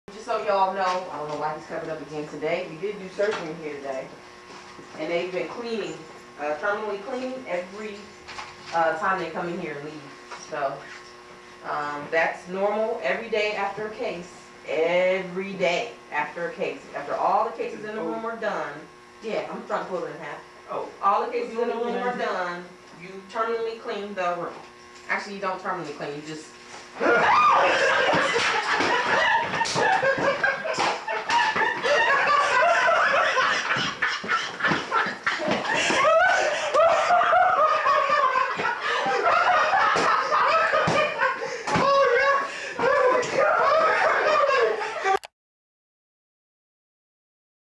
CuteWinFail: Dead Dummy
See a nurse freak out when a corpse comes back to life.